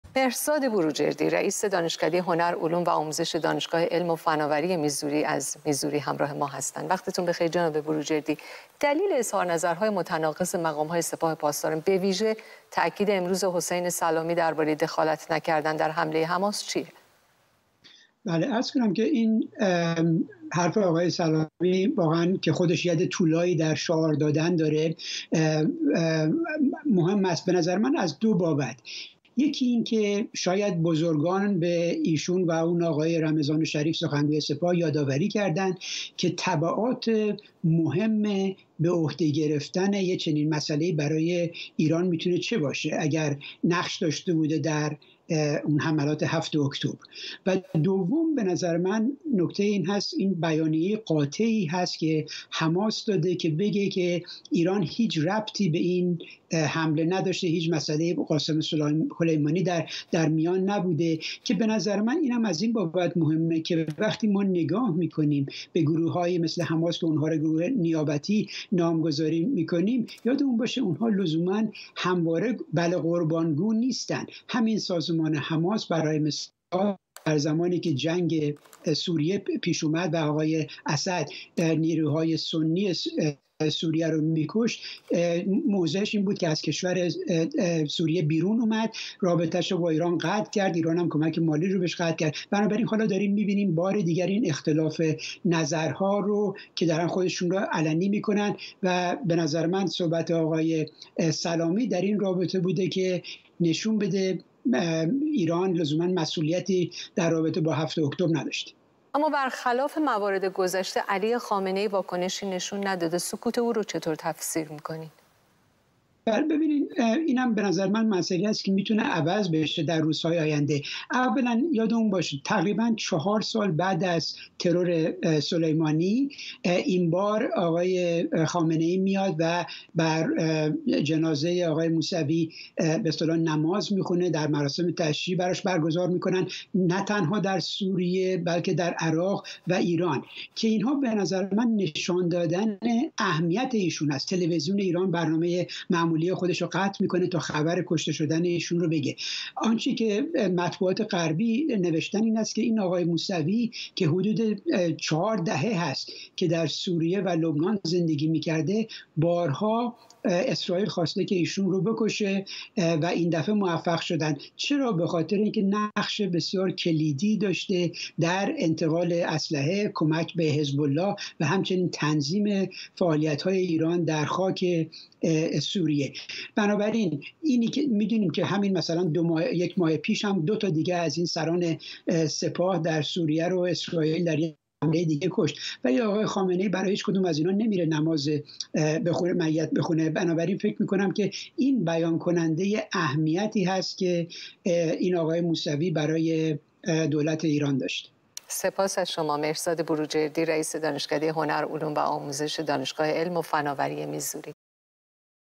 رد ادعای حمایت سپاه از حماس
حسین سلامی حمایت سپاه پاسداران از حمله حماس به اسرائیل را رد کرد.

گفت‌وگو با مهرزاد بروجردی، رئیس دانشکده هنر، علوم و آموزش دانشگاه علم و فناوری میزوری